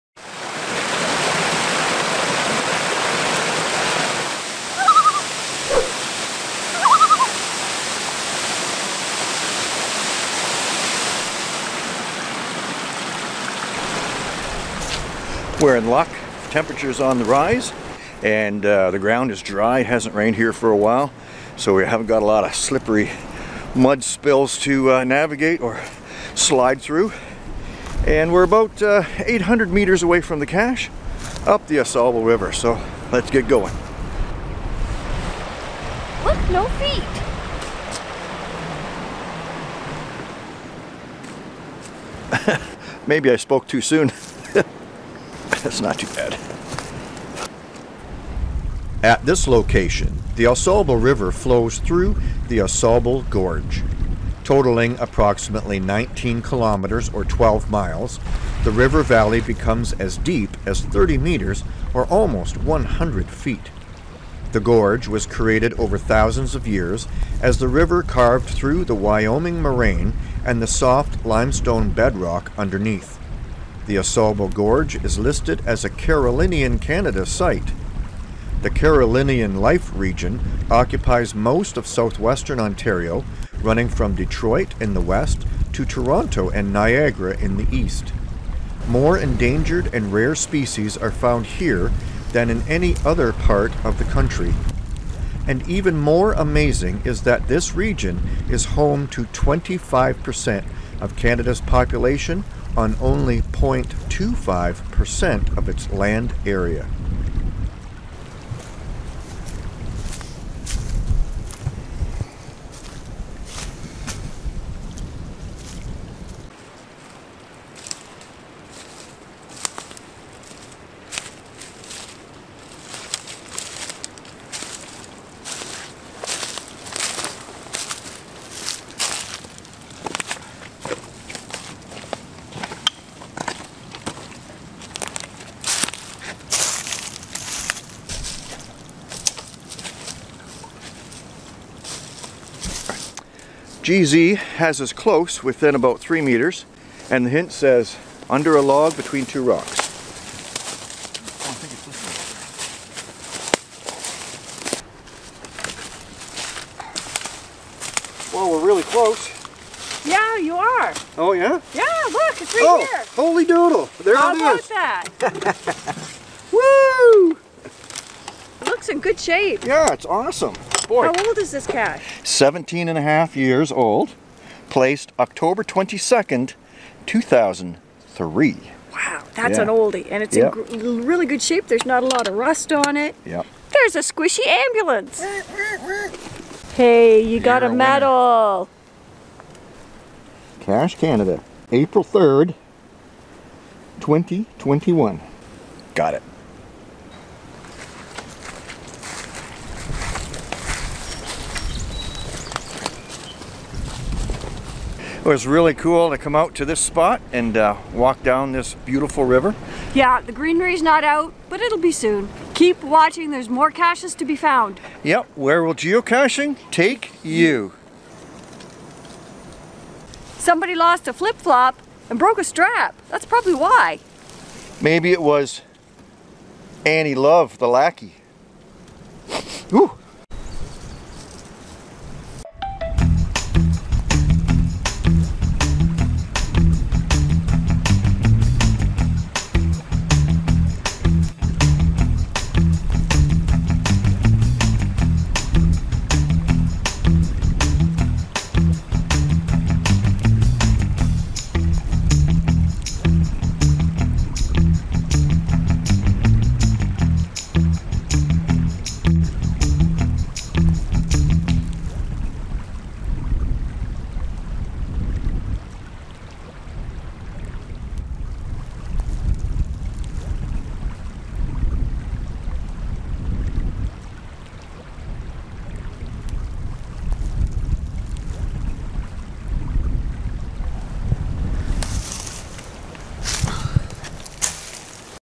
Geocaching down the river for 'Ausable Gorge' an Oldie but boy what a Goodie!
Ausable Gorge is the name of the cache and it takes its name from the place where it's at, the Ausable Gorge. One of southern Ontario's truly remarkable places. Come along with us on this journey for the cache and some amazing facts about the area.